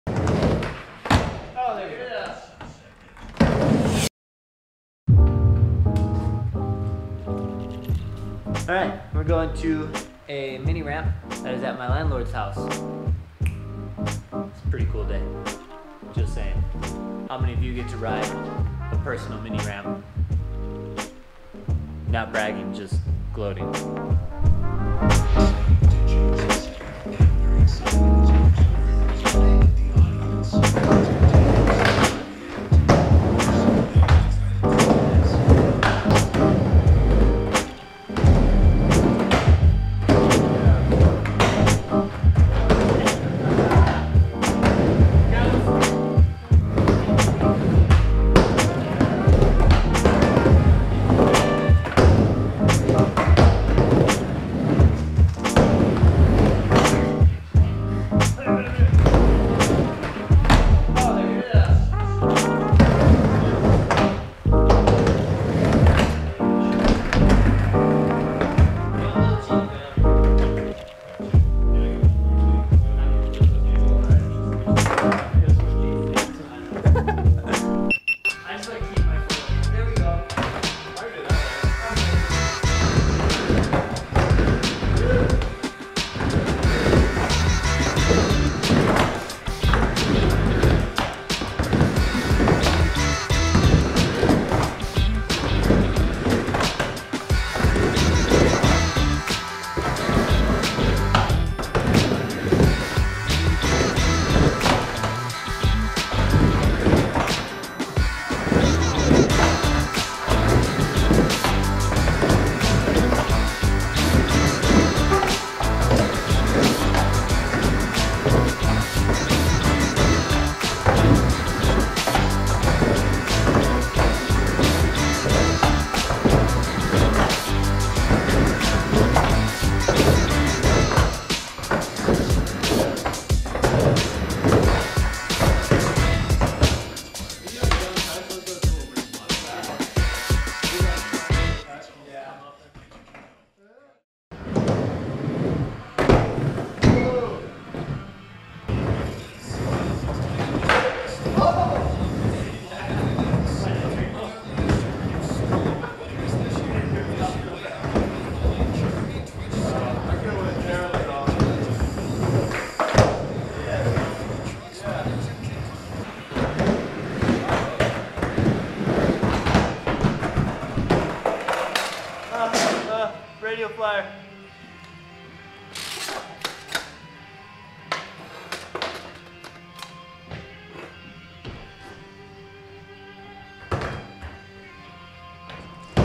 Private Indoor Mini-Ramp!!!
So @ReeseWillson built a Mini-Ramp inside of a huge barn and I decided it was time for me to ride it! I think I'm going to try and move it to my back yard!!!